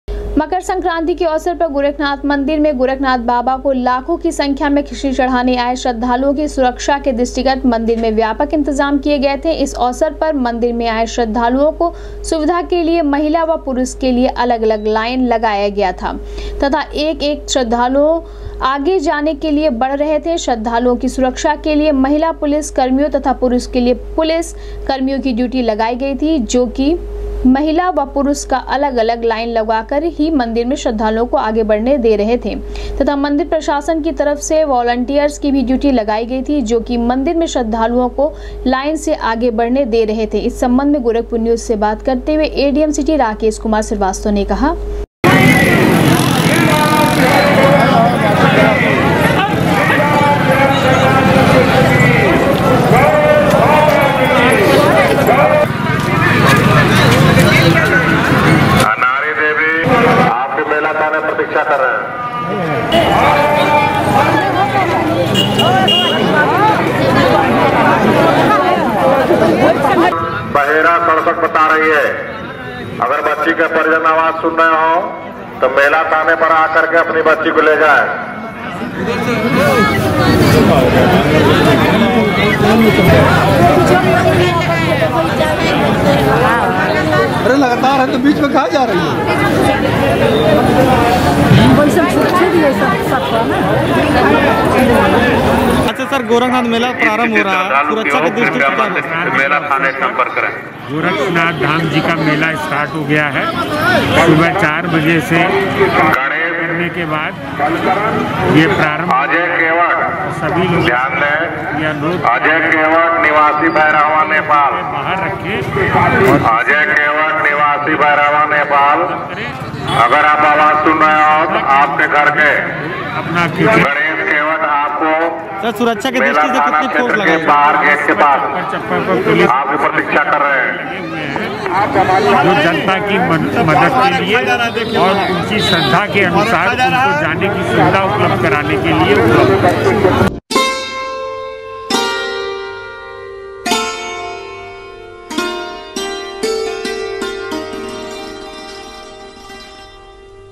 मकर संक्रांति के अवसर पर गोरखनाथ मंदिर में खिचड़ी चढ़ाने आये श्रद्धालुओं की सुरक्षा के दृष्टिगत रहे
मकर संक्रांति के अवसर पर गोरखनाथ मंदिर में खिचड़ी चढ़ाने आये श्रद्धालुओं की सुरक्षा के दृष्टिगत रहे व्यापक इंतजाम,समाज सेवी संगठन तथा पुलिस के जवान रहे मुस्तैद।।